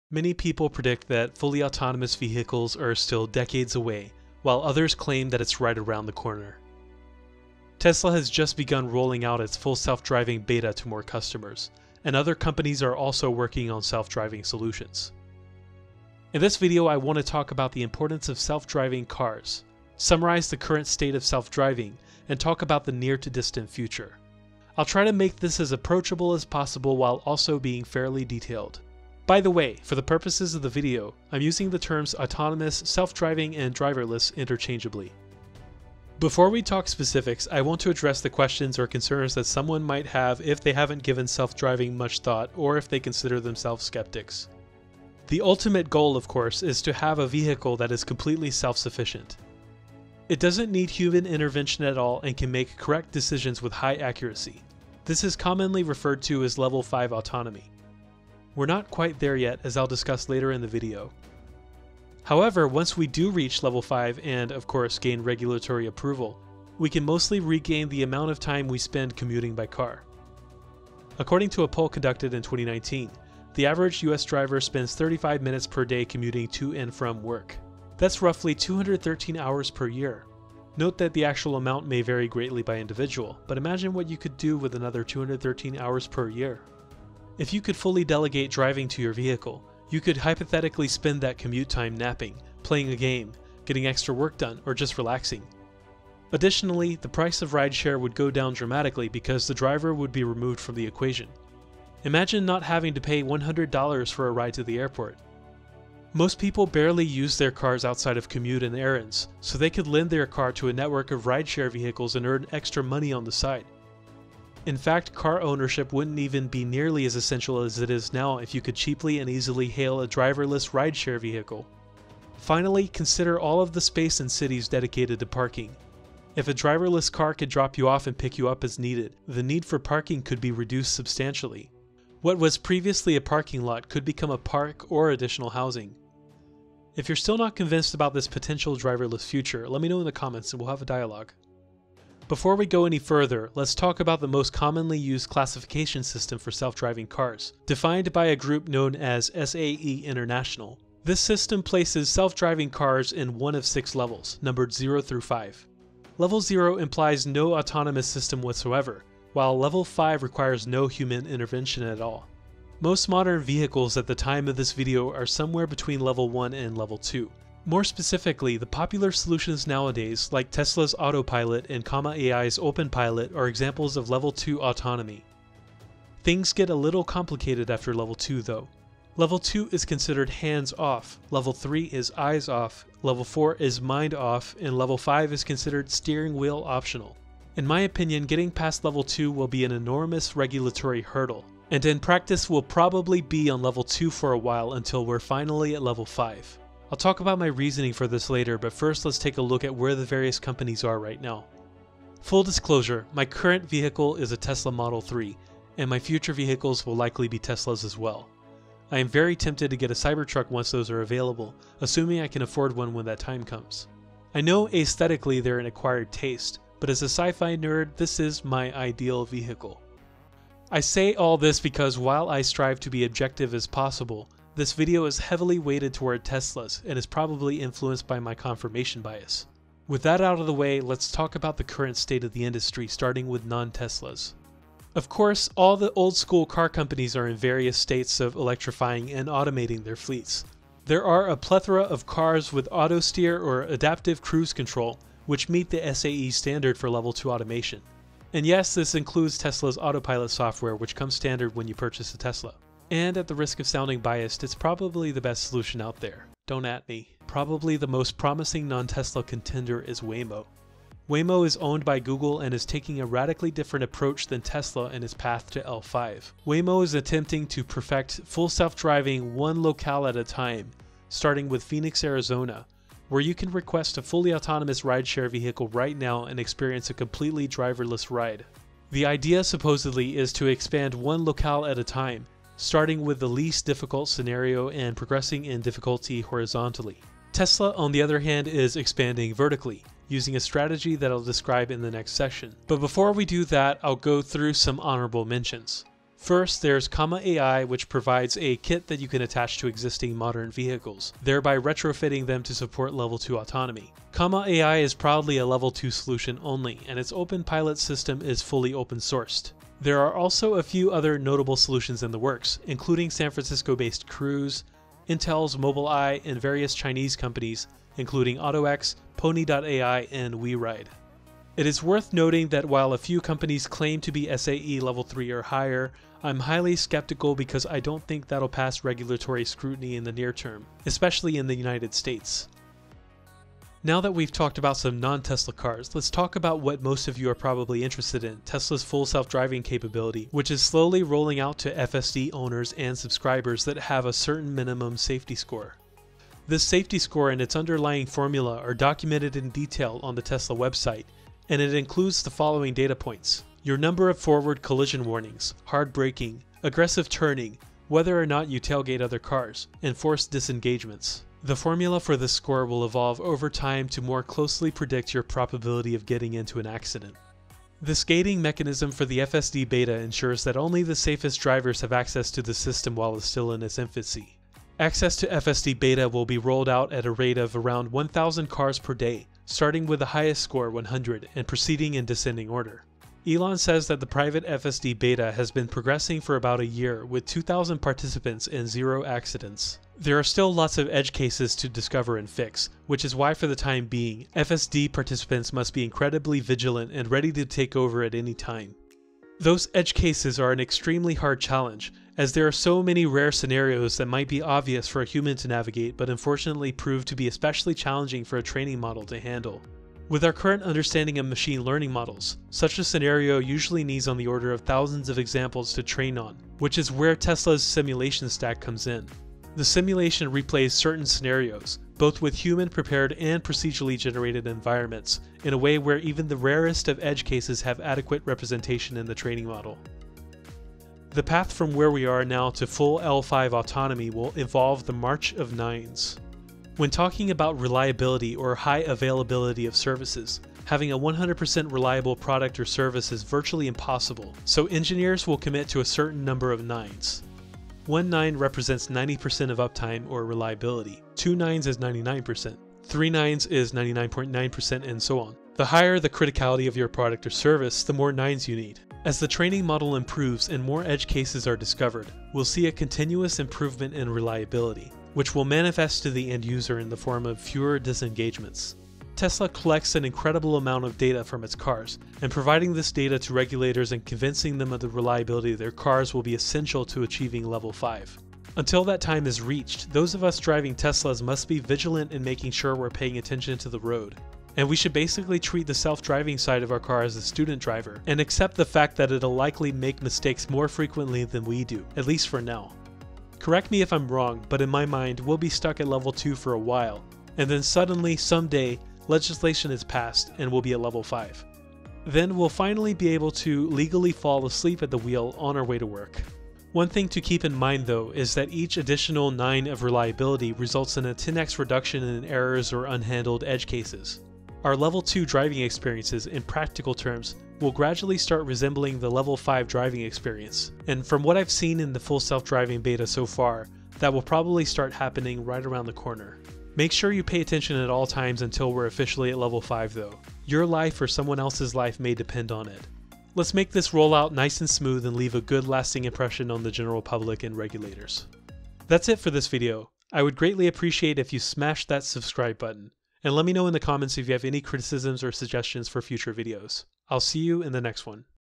Why Tesla's Full Self Driving is taking so long
In this video, I talk about the Tesla Full Self Driving (FSD) Beta, which is starting to roll out to those with a high Safety Score. FSD has been promised for a while, and it seems like it's just around the corner! How will Tesla get to Level 5? What even is Level 5? What's the current state in the rest of the autonomous vehicle industry? I answer all of these questions in the video.

Timestamps:
00:00 - Intro
00:35 -Why self driving?
02:21 - Levels of autonomy
03:21 - Current state: Tesla and others
05:33 - Tesla's path to Level 5
08:58 - Outro